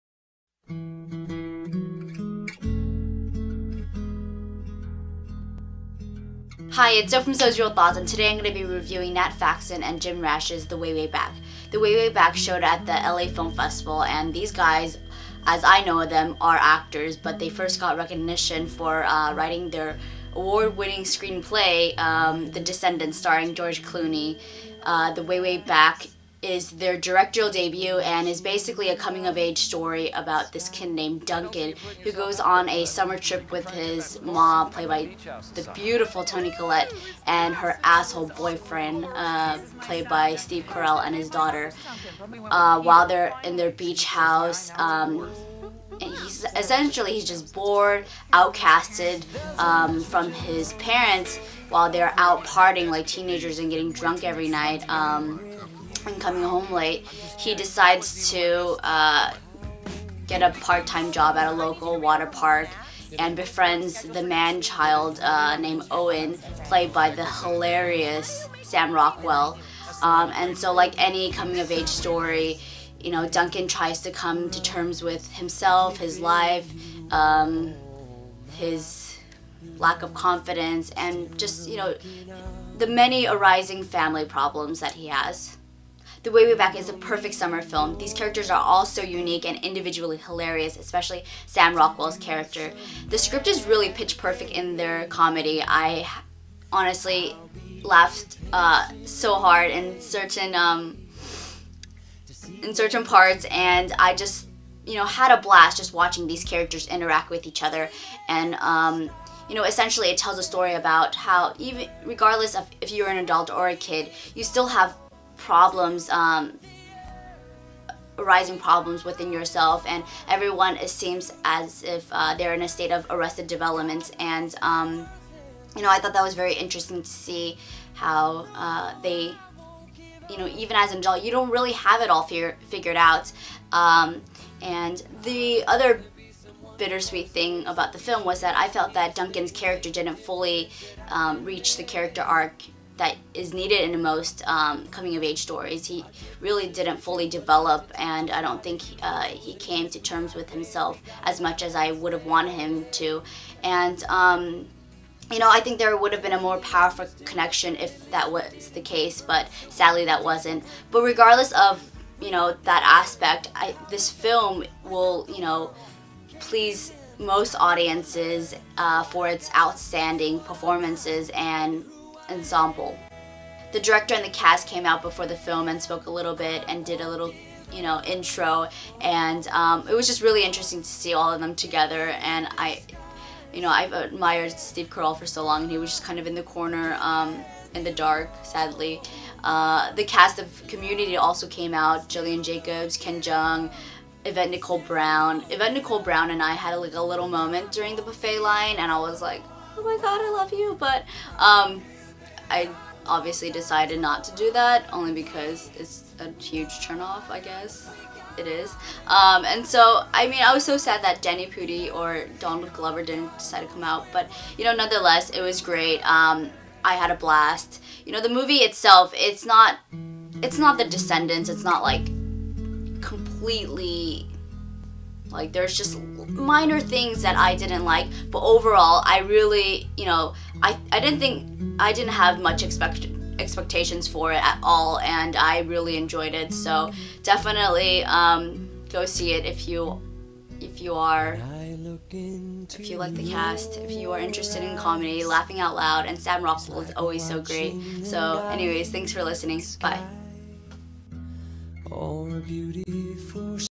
LAFF '13: The Way, Way Back (2013)
The Way, Way Back is probably the best summer comedy  film of this year! It's super quirky and fun and critics and audiences alike will be able to enjoy this one. There are minor faults but it's still a good old time!